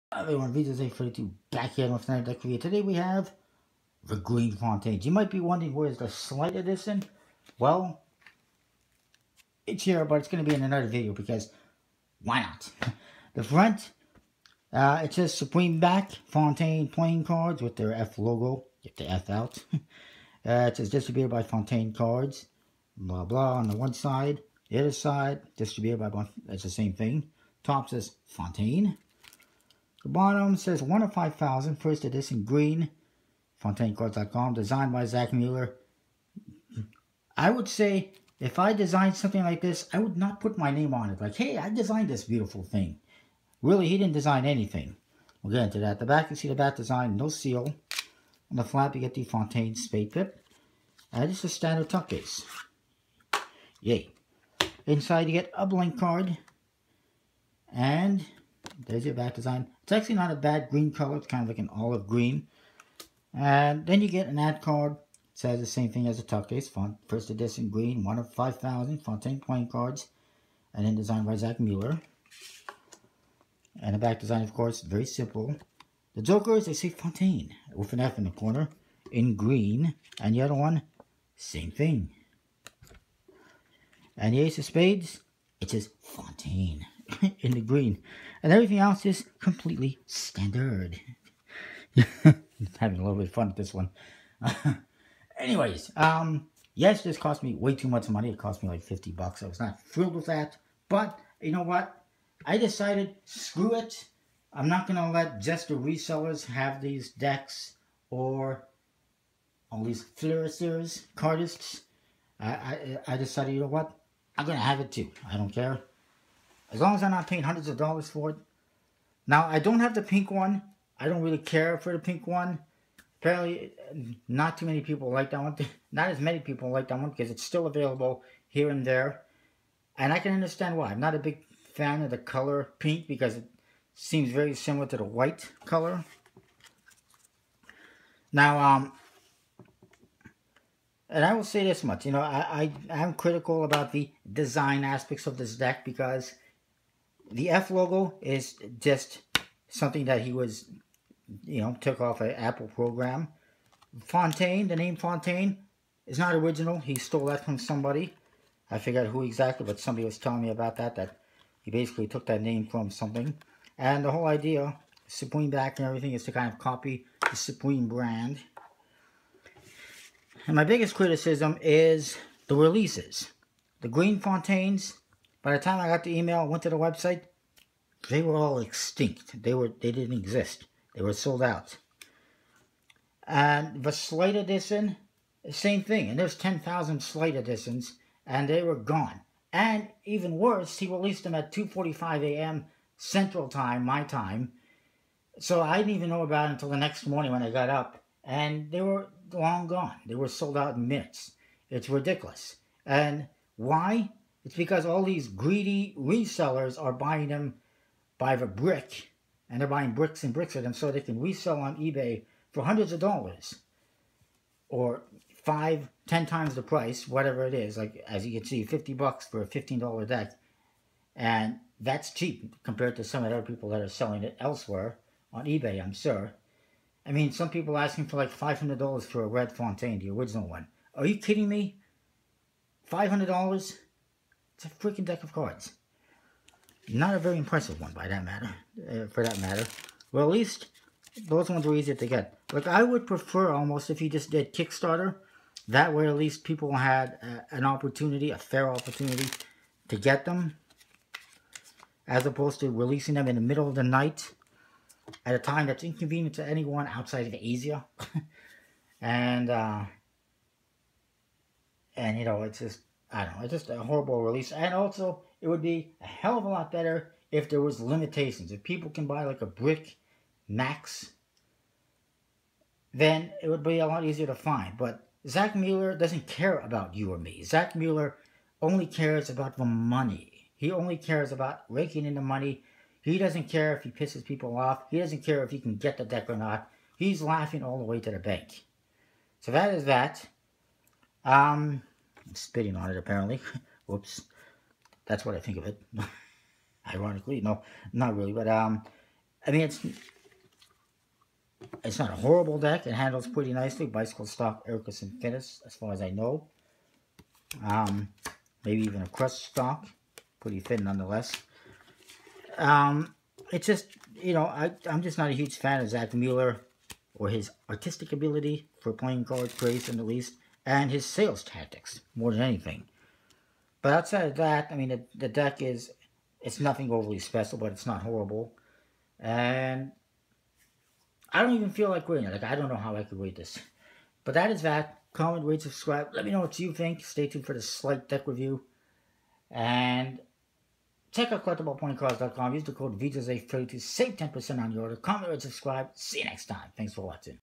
Fontaine Green Deck Review
It's an okay deck but getting it is a pain.

For the best playing cards check out CollectablePlayingCards.com,

                                               - vic10 for $10 off any membership